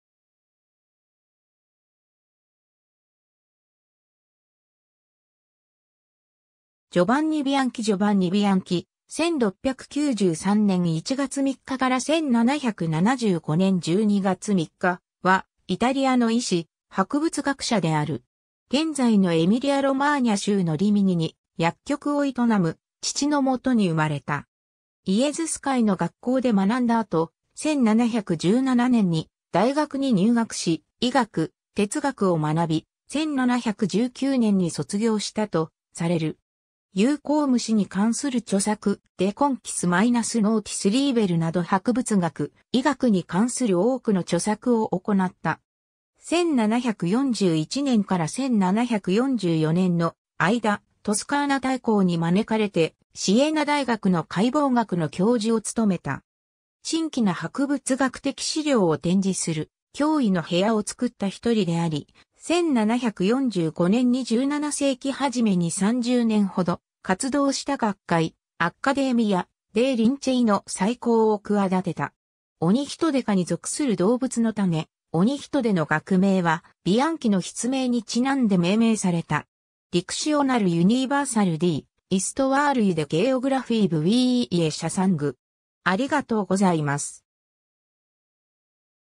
ジョヴァンニ・ビアンキ
ジョヴァンニ・ビアンキ ジョヴァンニ・ビアンキ（Giovanni Bianchi、筆名：ヤーノ・プランコ（Jano Planco、Janus Plancus）、1693年1月3日 - 1775年12月3日）はイタリアの医師、博物学者である。
 現在のエミリア＝ロマーニャ州のリミニに、薬局を営む父のもとに生まれた。
イエズス会の学校で学んだ後、1717年に大学に入学し、医学、哲学を学び、1719年に卒業したとされる。
有孔虫に関する著作、"De Conchis minus notis liber" (1739)など博物学、医学に関する多くの著作を行った。
1741年から1744年の間、トスカーナ大公に招かれて、シエナ大学の解剖学の教授を務めた。
 珍奇な博物学的資料を展示する「驚異の部屋」を作った一人であり、1745年に17世紀初めに30年ほど活動した学会、アッカデーミア・デイ・リンチェイの再興を企てた。
 オニヒトデ科に属する動物の種、オニヒトデ（Acanthaster planci）の学名はビアンキの筆名に因んで命名された。
 Dictionnaire universel d’histoire et de géographie Bouillet Chassang